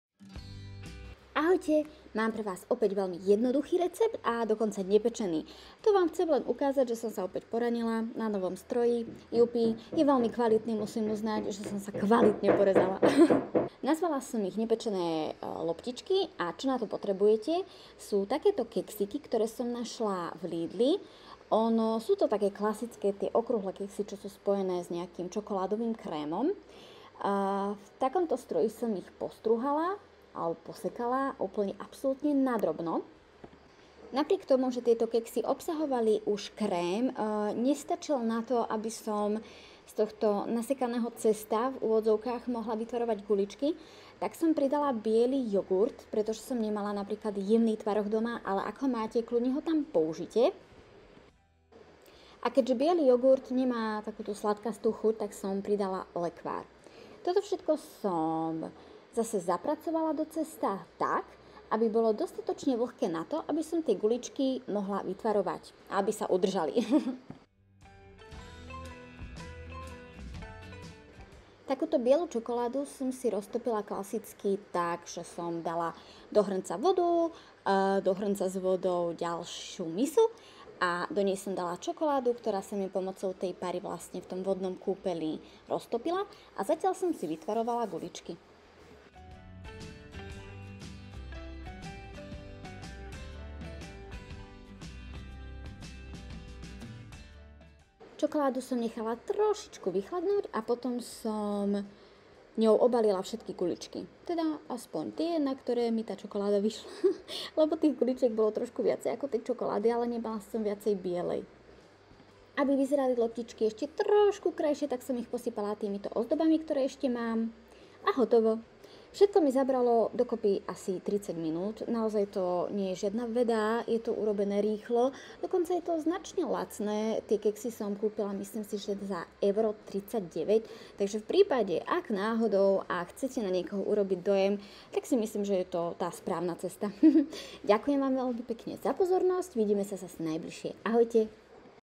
Nepečené loptičky
Ahojte,
keďže sa Vám veľmi páčili Čoko loptičky, na ktoré máte link konkrétne tu chcela som vymyslieť niečo ešte ešte jednoduchšie, i keď to spočiatku vyzeralo, že už to jednoduchšie ani neide :)
Ale predsa.
Čo Vám treba?
Konkrétne asi 30 minút času a:
- keksíky (jedno akej značky, ale teda ja som robili také čo sú spájané s krémom),
- 1-2 lyžice (ja som síce dala biely jogurt), no uprednostnila by som jemný tvaroh :-)
- 1 lyžicu lekváru (Ak by ste dali viac tvarohu, ten lekvár nebude ani potrebný, no keďže biely jogurt nemá sladkú chuť, pridala som lekvár ako sladidlo),
- viem si do cesta predstaviť dať aj mleté orechy (i ked tieto mali aj tak orechovú príchuť vďaka krému, ktoré pôvodné keksy v sebe mali.

Všetko si musíme zapracovať v mixéri do vláčnejšej masy, aby bola dosť pevná na to, aby sme ju vedeli tvarovať.
Ja som teda zvolila klasické guličky :)
Rozpustila som si (tentokrát) bielu čokoládu ale použite kľudne čiernu a ozdobila som cukrárskymi ozdobami :-)
Tu sa tak isto fantázii nekladú, ponúkam Vám len obraz toho ako si ich môžete upraviť pre svoje potreby a možnosti :-)
Myslím, že sú také nenáročné a zároveň také zaujímavé a chutné, že nimi nikoho neurazíte :)
Dobrú chuť a zábavné tvorenie.
Monika